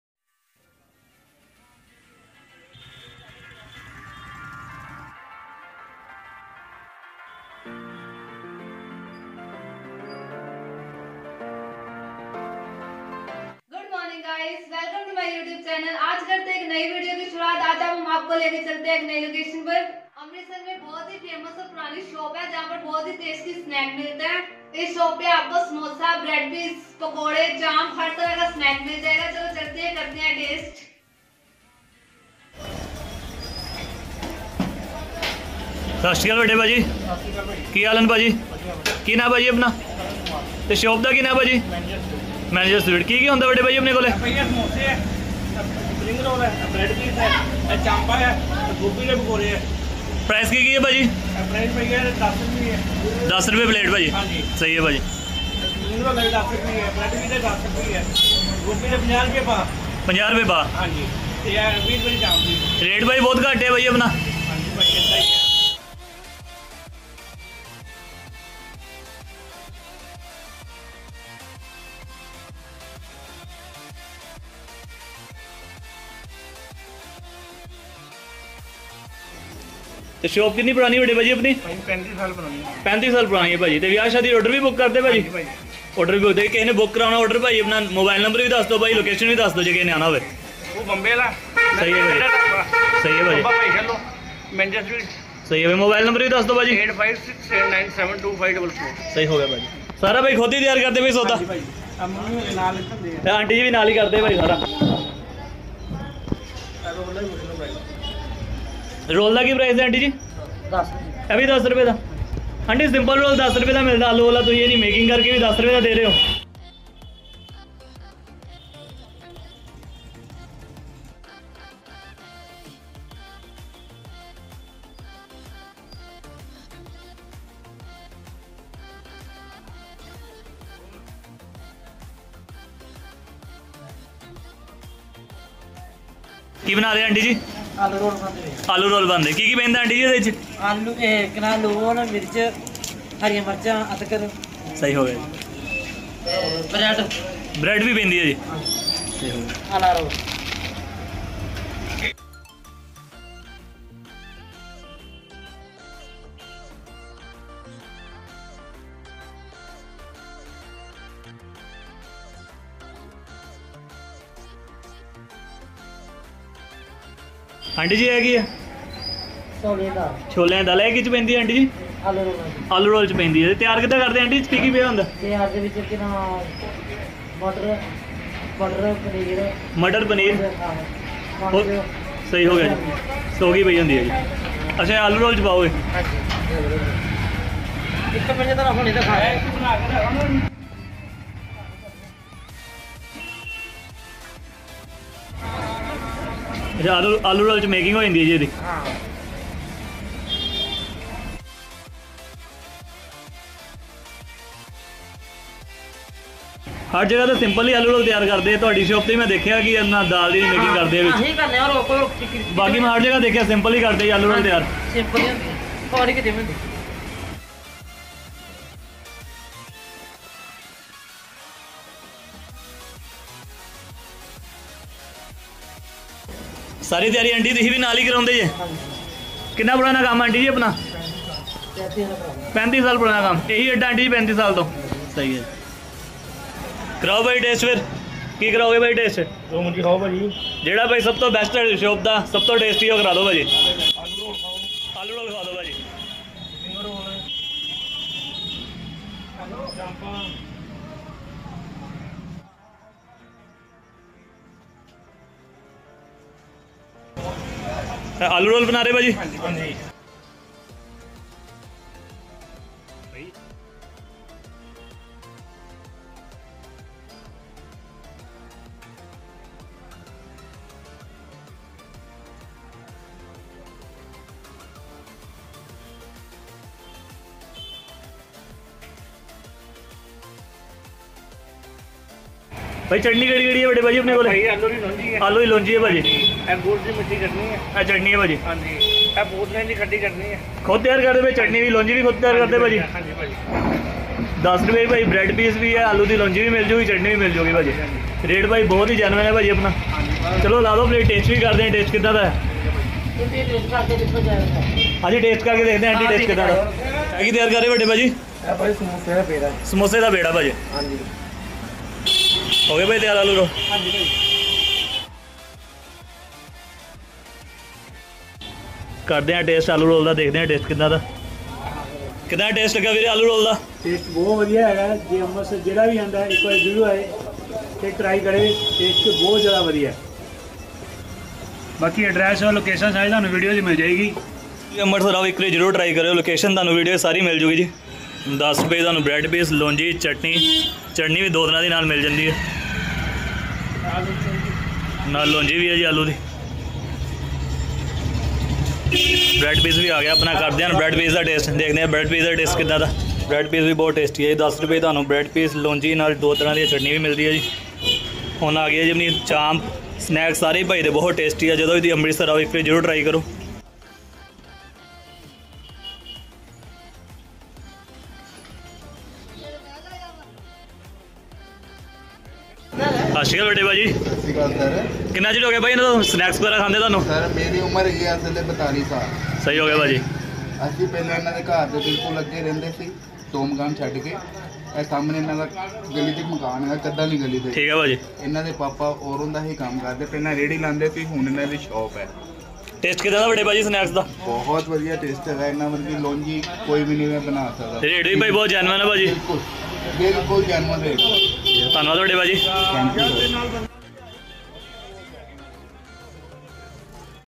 MANGER SWEETS 35 YEARS OLD SHOP IN AMRITSAR 😍 SAMOSA😋CHAAP😋SPRING ROLLS😋FAMOUS SHOP IN AMRITSAR 😋😋
MANGER SWEETS 35 YEARS OLD SHOP IN AMRITSAR

SAMOSA 😋 SPRING ROLLS 😋 CHAAP 😋

FAMOUS AND OLD SNACKS SHOP IN AMRITSAR !

LOCATION  : GALI LAMBA WALI,  GURUDWARA TOBHA BHAI SHALO JI AMRITSAR PUNJAB !!

DEEP KAUR

YOUTUBE  CHANNEL : DEEP LOVE MOM